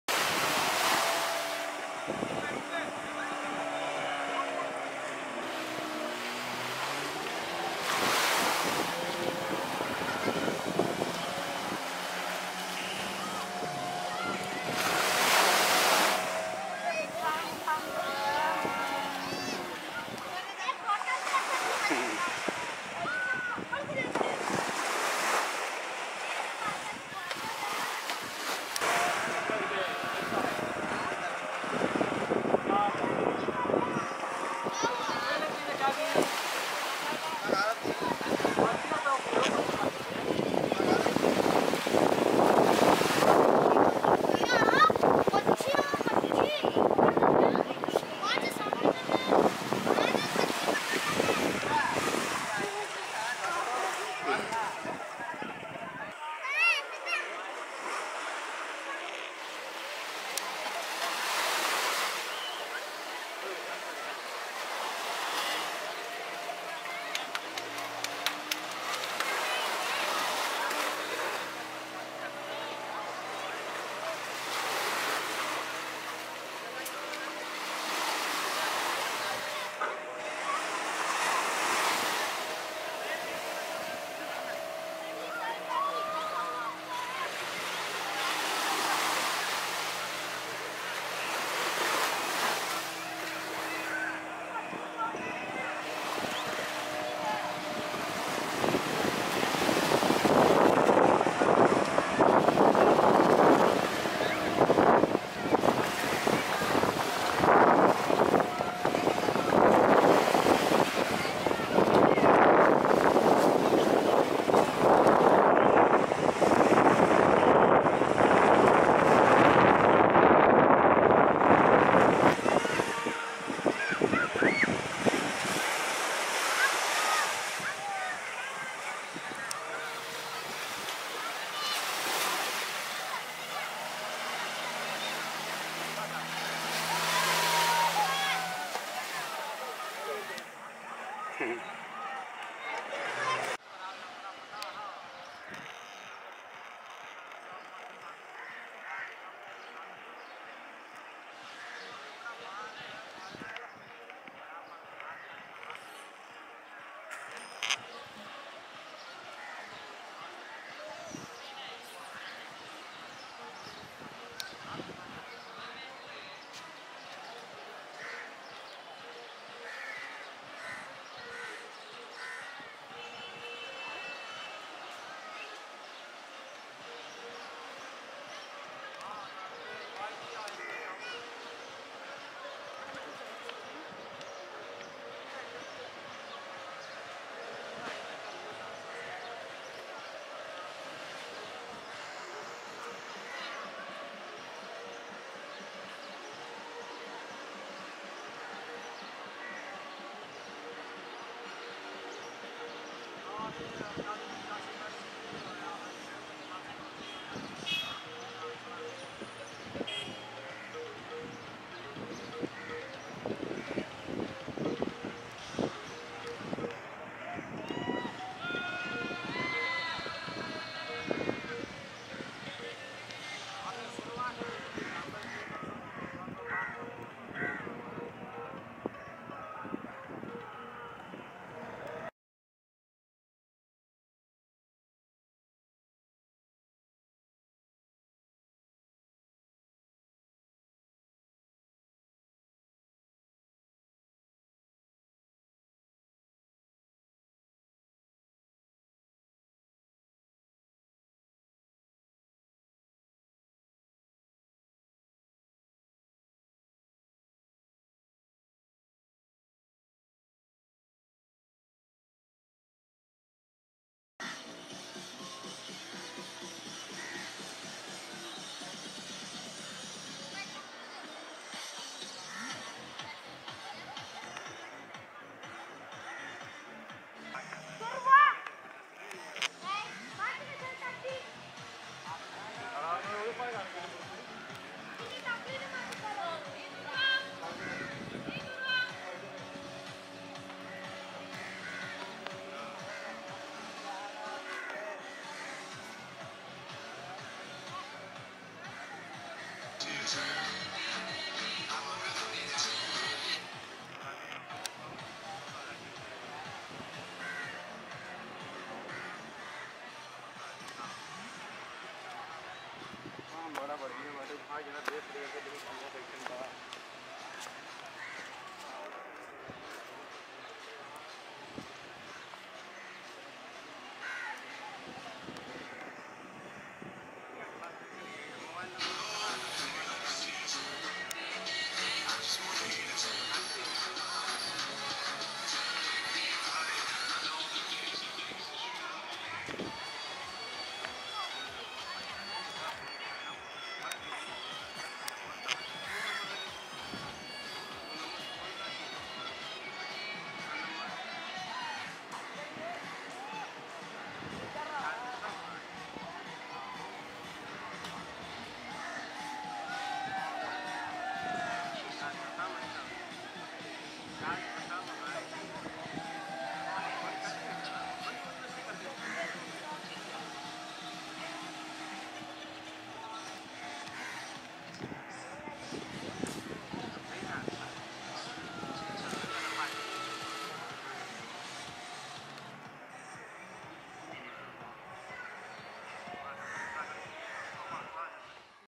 Banana boat ride
Banana ride at Baina beach, Goa on 4.1.2022 Vasco da Gama